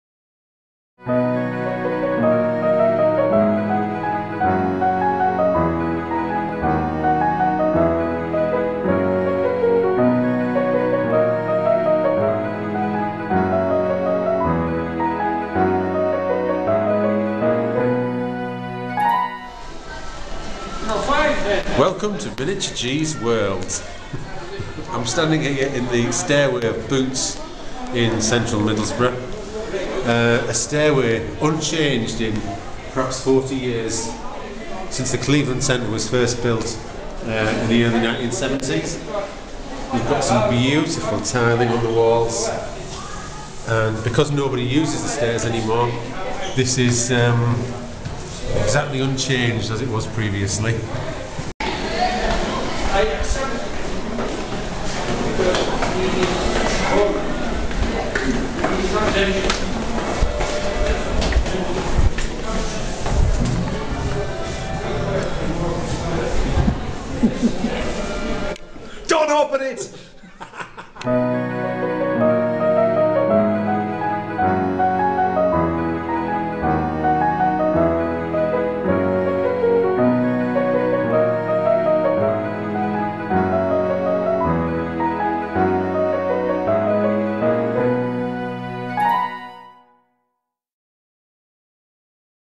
Miniature G's World Episode 1 - The Back Stairs in Boots
Intrepid explorer Miniature G travels the world looking for undiscovered wonders. This week - the back stairwell of the Boots store in Middlesbrough.